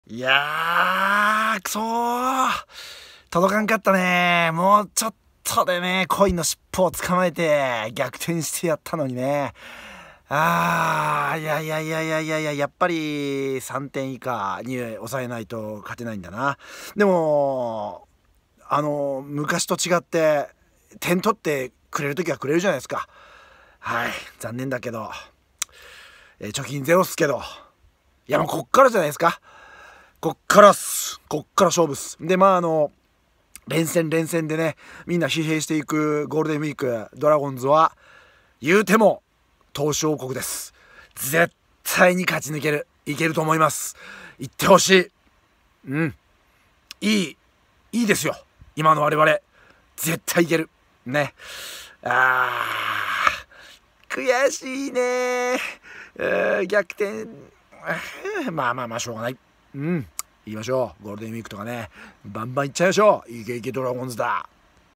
4/27 中日x広島 4-6#梅津晃大 次っすよ！カリステ と 細川さんでばこーん！ 戦えてます！あと少しだった！貯金0 でも 俺たちここからだよね！
4/27  中日x広島 4-6
梅津晃大　次っすよ！
カリステ　と　細川さんでばこーん！　戦えてます！あと少しだった！
貯金0 でも  俺たちここからだよね！

※ここはただドラゴンズの勝利を共に喜び、分かち合うためだけの場所です！